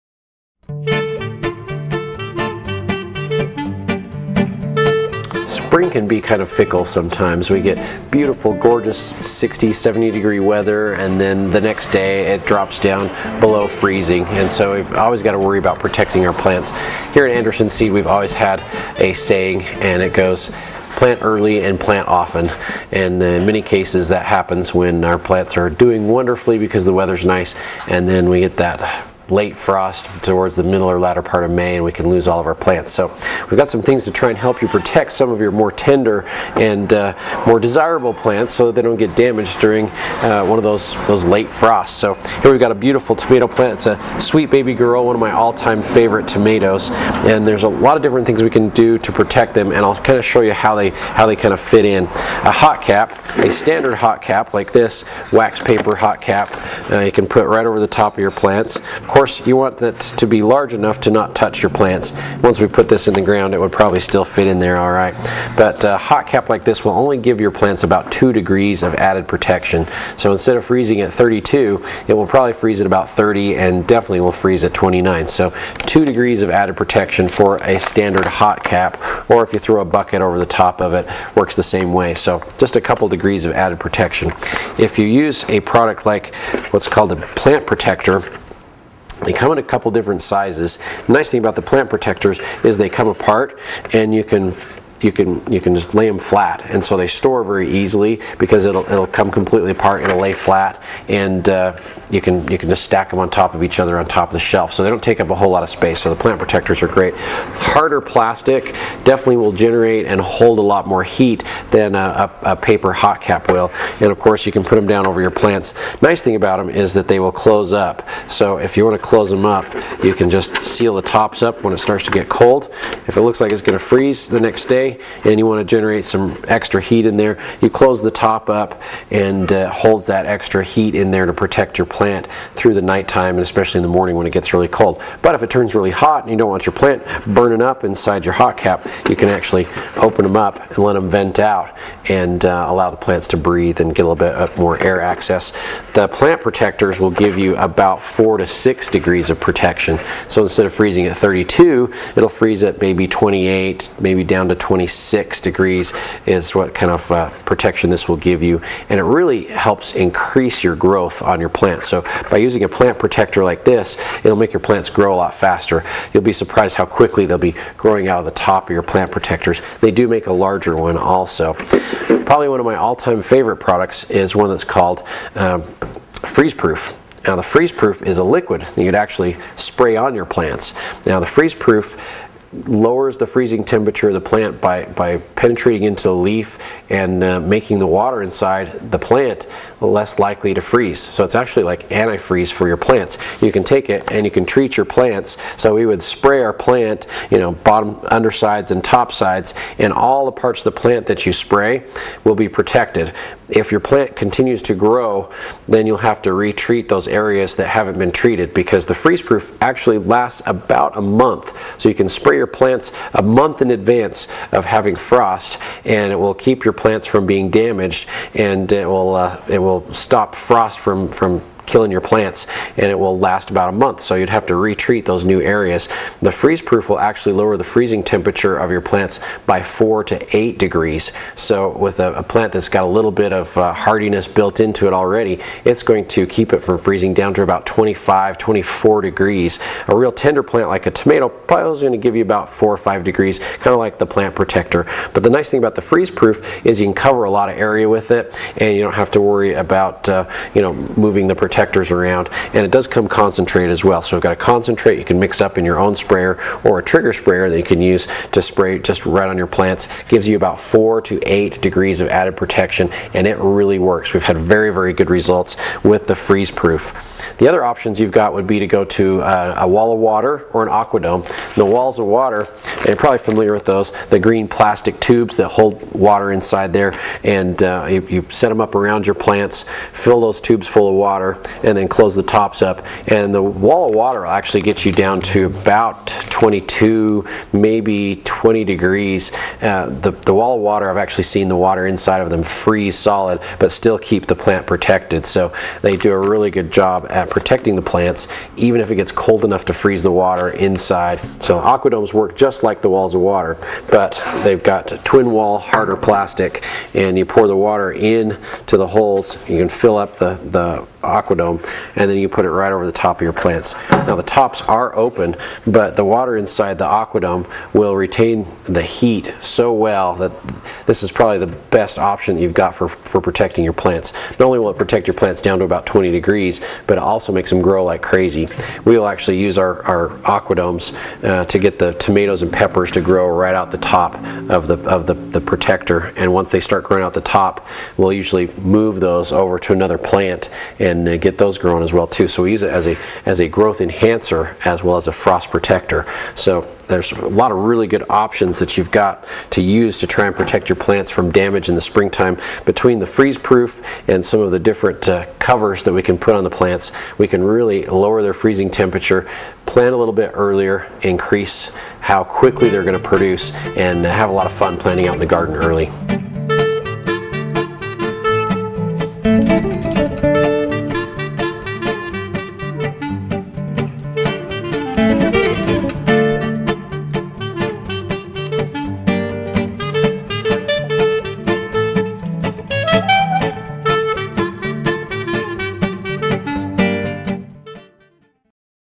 Anderson Seed and Garden What are the best ways to protect against cold damage?
From Hot Caps to Aqua Domes, Mark explains the benefits and limitations of multiple types of plant protectors made to resist frost damage.  Freeze proof, Season Starters, and Aqua Domes give the most protection of all, at reasonable prices.